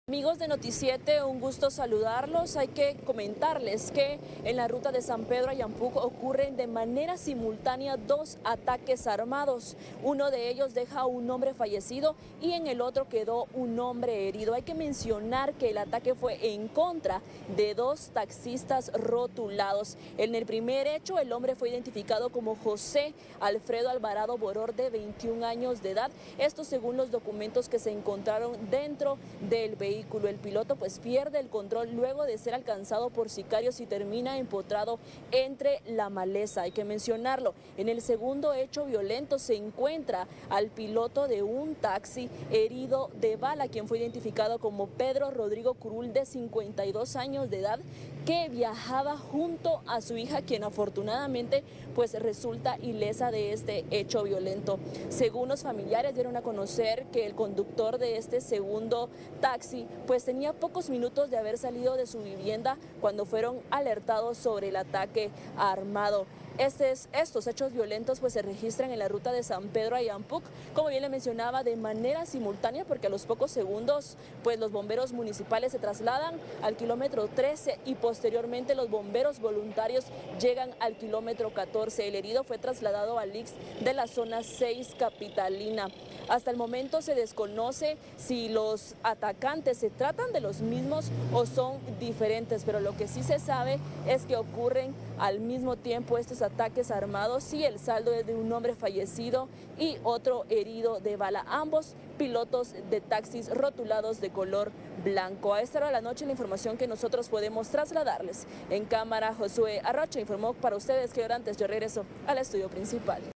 Un taxista fue asesinado en ruta hacia San Pedro Ayampuc
La víctima quedó dentro del automotor

Sobre la ruta hacia San Pedro Ayampuc, se registró un ataque armado contra el conductor de un taxi.